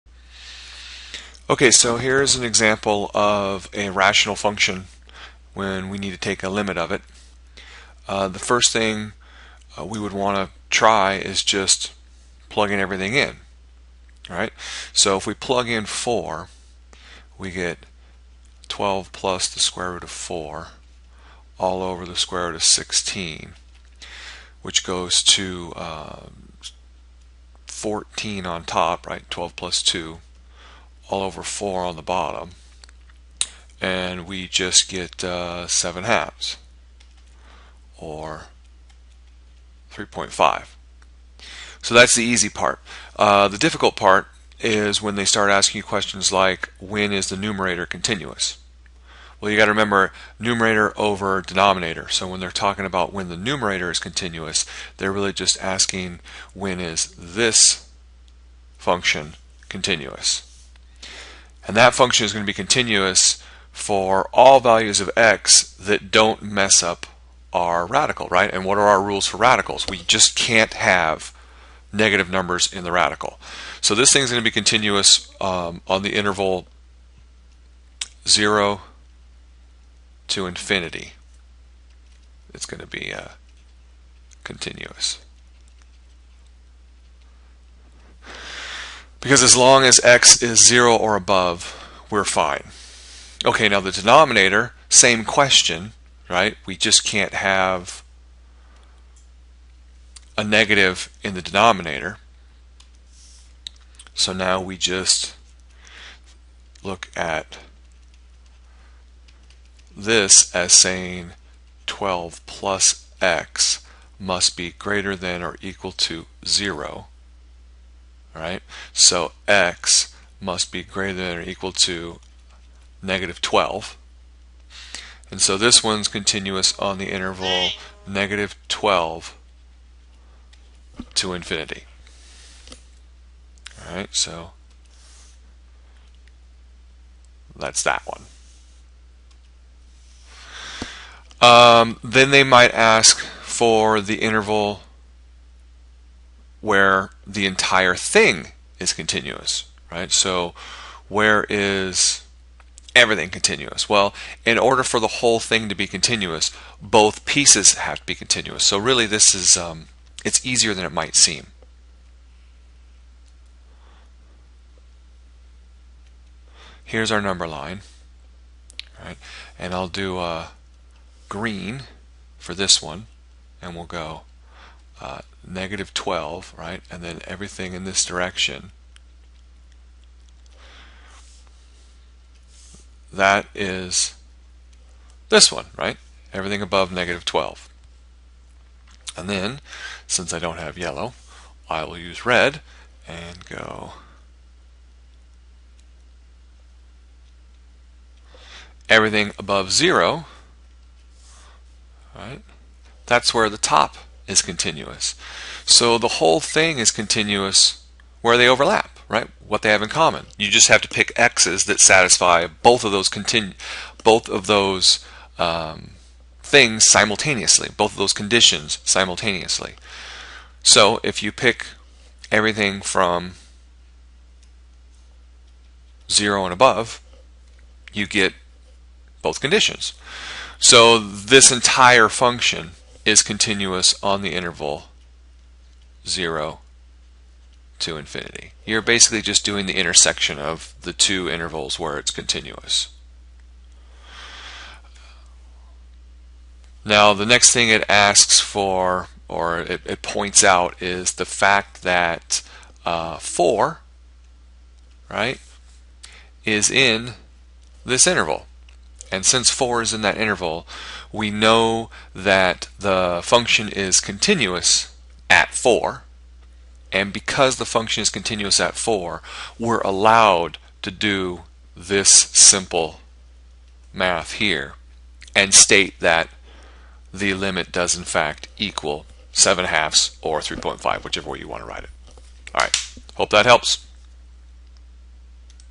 Calculus - Continuity - Limit of a Rational Function
Looking at the continuity of both the numerator and denominator in order to determine the overall interval of continuity for the function in order to determine if we can evaluate the limit at a given point "a" or not.